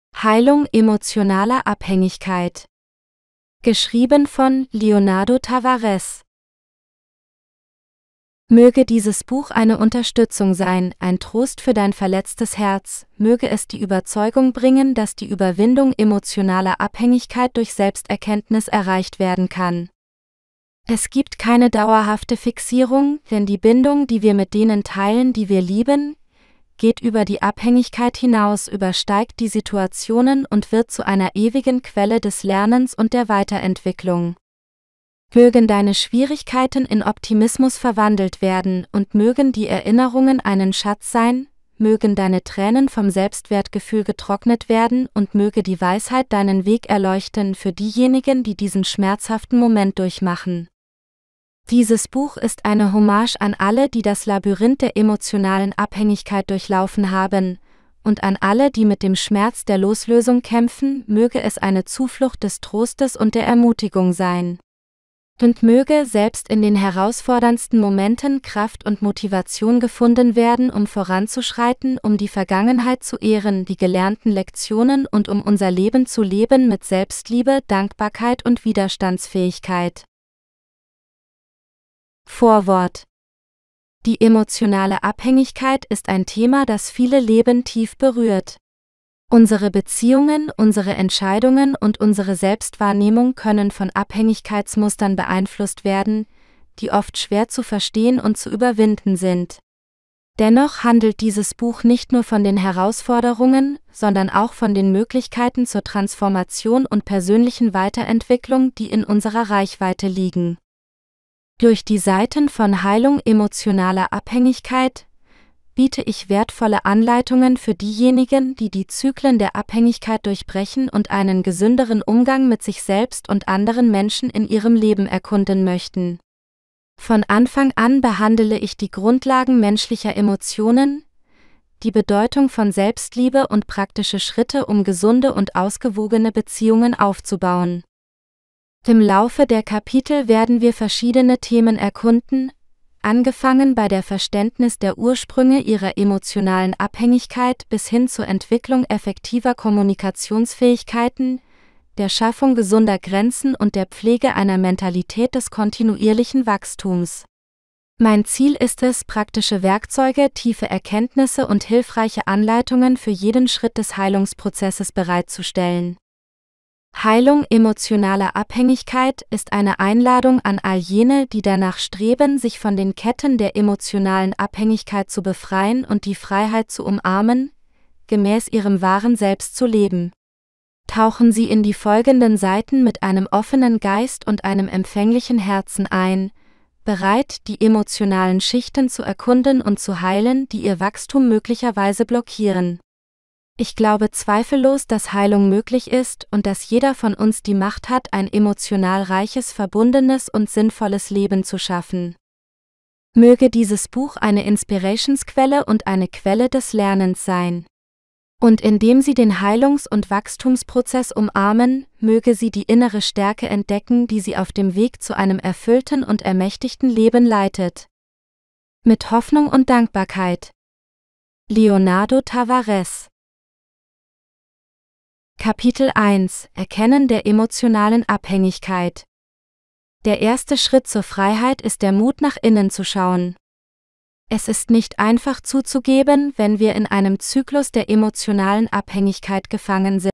Heilung emotionaler Abhängigkeit - Leonardo Tavares | Hörbuch | Vorschau
Emotionale Abhängigkeit ist ein Thema, das viele Menschen auf tiefgreifende Weise betrifft. Unsere Beziehungen, unsere Entscheidungen und unsere eigene Selbstwahrnehmung können von Abhängigkeitsmustern beeinflusst werden, die oft schwer zu verstehen und zu überwinden sind.

"Heilung emotionaler Abhängigkeit" ist jedoch nicht nur ein Buch über die Herausforderungen, sondern auch über die Möglichkeiten der Transformation und persönlichen Entwicklung, die in unserer Reichweite liegen.

Im Laufe der Kapitel erkunden wir verschiedene Themen, um die Ursprünge Ihrer emotionalen Abhängigkeit zu verstehen, effektive Kommunikationsfähigkeiten zu entwickeln, gesunde Grenzen zu setzen und eine kontinuierliche Wachstumsmentalität zu pflegen. Tauchen Sie in die folgenden Seiten mit einem offenen Geist und einem empfänglichen Herzen ein, bereit zu verstehen und die emotionalen Schichten zu heilen, die Ihr Wachstum möglicherweise blockieren.

Die Heilung ist möglich, und jeder von uns hat die Kraft, ein emotionales, verbundenes und sinnvolles Leben zu erschaffen.